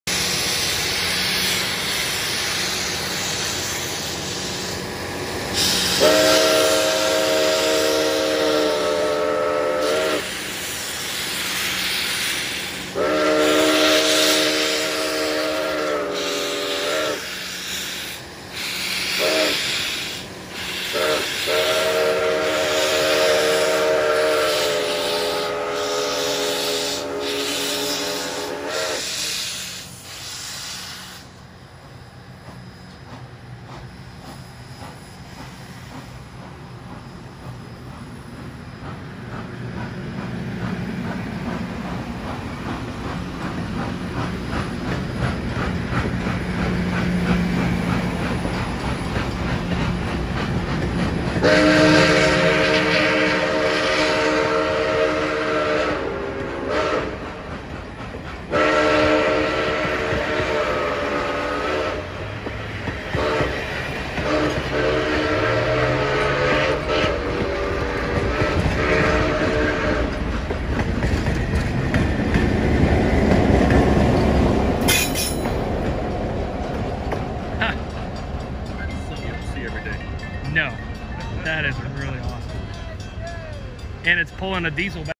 Steam engine Locomotive￼ train. Angola Indiana at whiskey and wine event!￼
The New Haven Indiana railroad club brought their steam engine locomotive up to Angola Indiana to their whiskey and ice cream event in July 2022. This is one of many more that they will be doing in Angola next to Trine University￼￼.